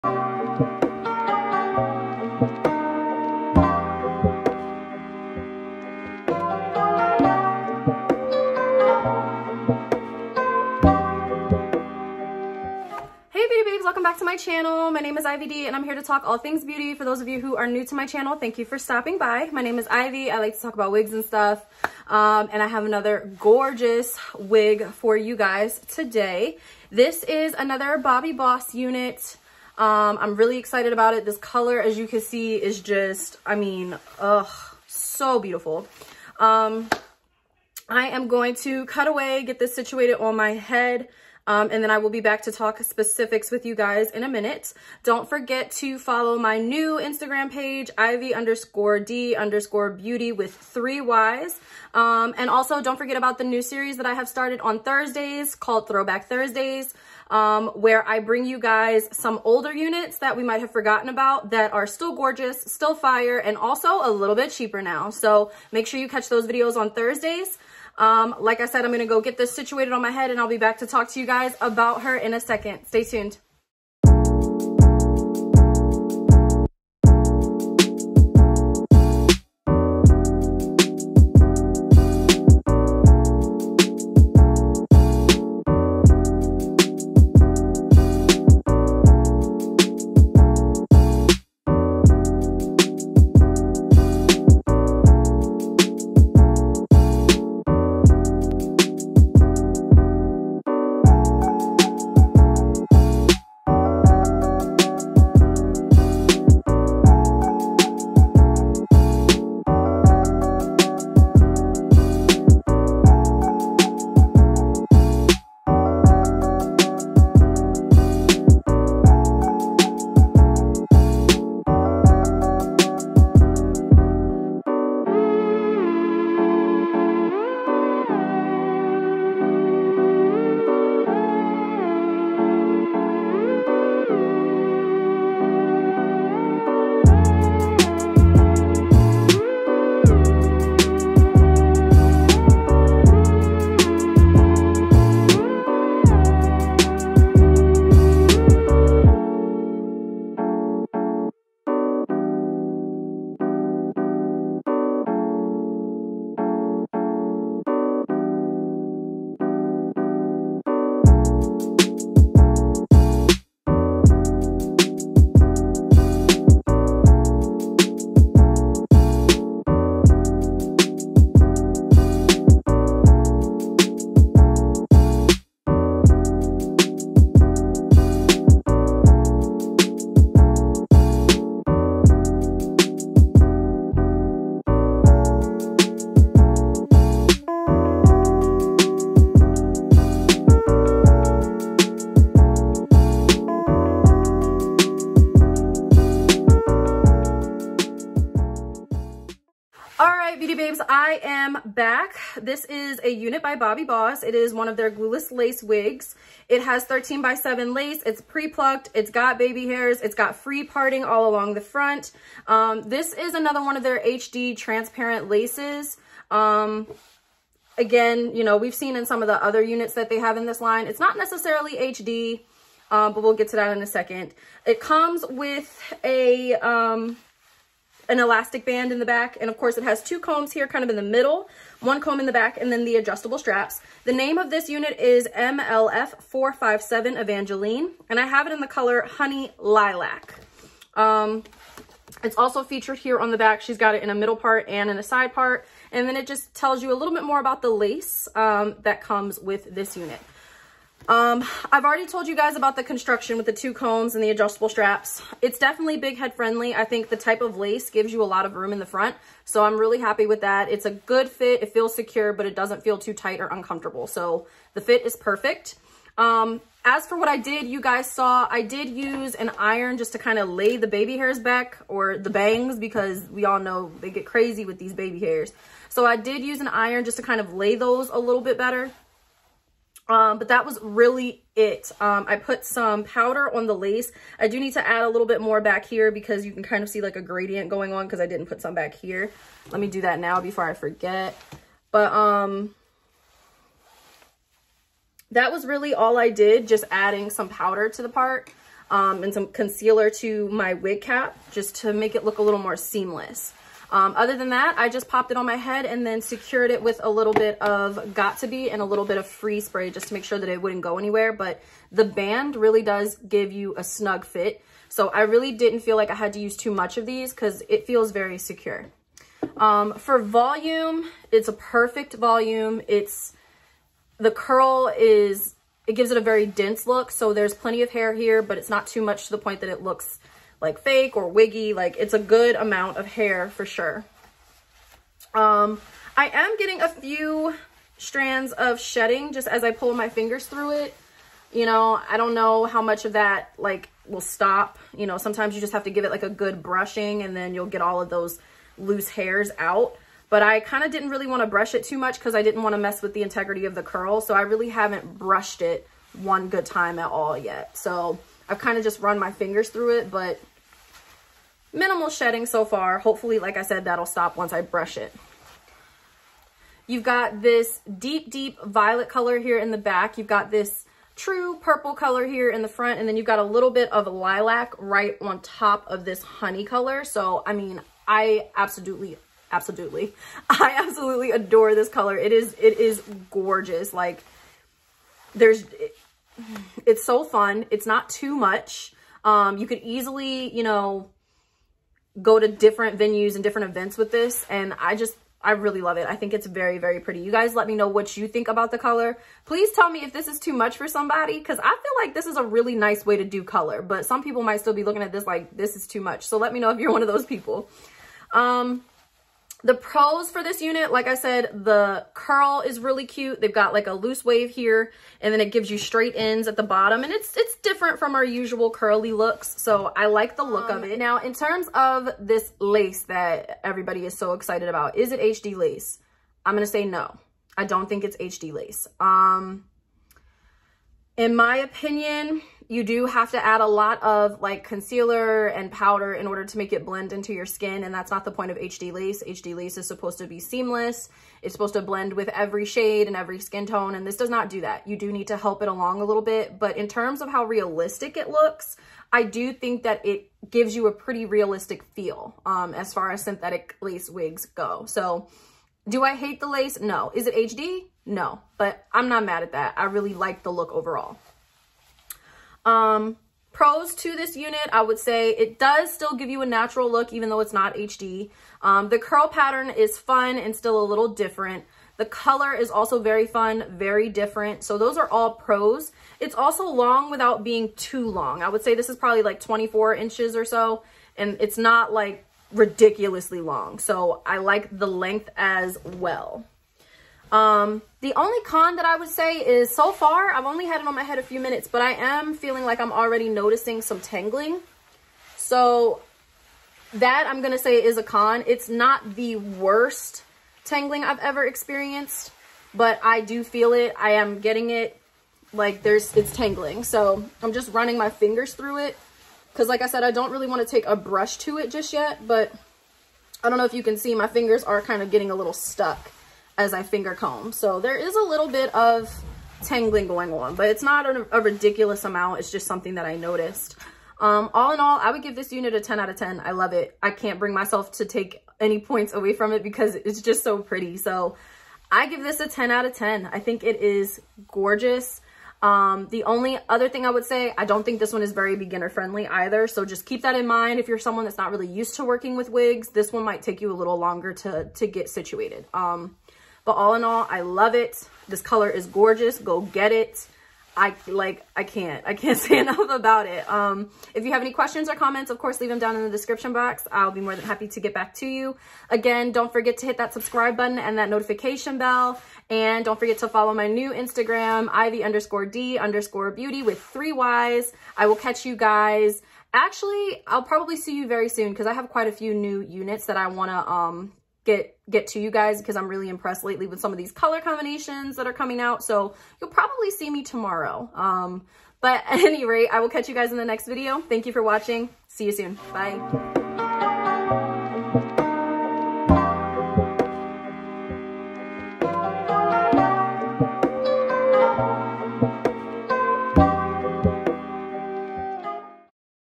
IN LOVE!!! Colorful HD Lace Unit Evangeline BOBBI BOSS DID IT AGAIN!
💖Social Media💖

💖 Hey BeautyBabes! Bobbi Boss did it again! This color is LIFE! Let me know what you think about this unit in the comment section down below!💖

💖SPECS💖
Style: MLF457 Evangeline
Brand: Bobbi Boss
Color: Honey Lilac
Big head friendly
Heat safe

💖Recent Reviews!💖